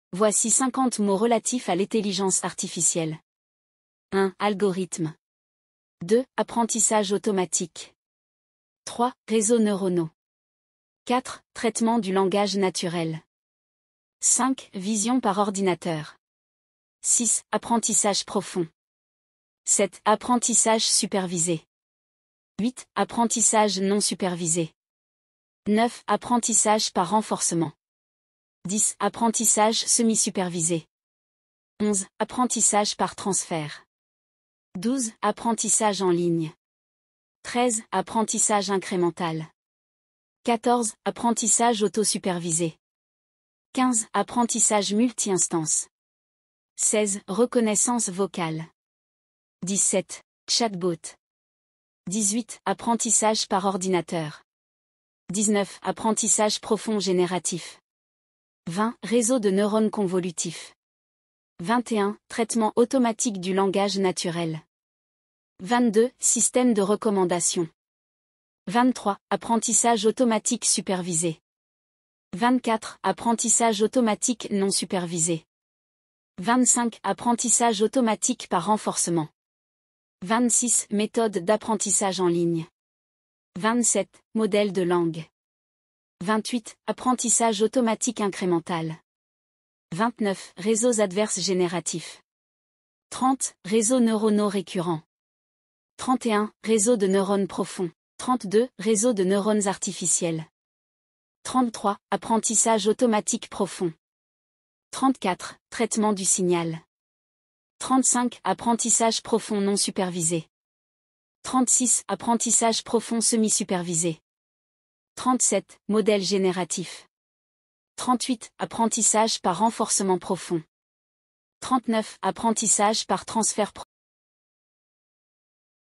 50 mots relatifs à l'intelligence artificielle @françaisauthentique#françaisfacile
Dans cette vidéo passionnante, nous vous présentons 50 mots clés relatifs à l'intelligence artificielle (IA). Plongez-vous dans le fascinant domaine de l'IA et explorez les termes et concepts qui façonnent notre compréhension de cette technologie révolutionnaire.

Que vous soyez novice ou passionné d'IA, cette vidéo vous offre une occasion unique d'approfondir vos connaissances. Nous passerons en revue des mots tels que l'apprentissage automatique, les réseaux neuronaux, l'apprentissage profond, la vision par ordinateur, le traitement du langage naturel, les robots autonomes, l'apprentissage supervisé, l'apprentissage non supervisé, l'éthique de l'IA, l'apprentissage par renforcement, les données massives et bien d'autres encore.

Préparez-vous à être immergé dans l'univers passionnant de l'intelligence artificielle et à enrichir votre vocabulaire technique. Que vous soyez un professionnel de l'IA, un étudiant ou simplement curieux de cette technologie en plein essor, cette vidéo est faite pour vous !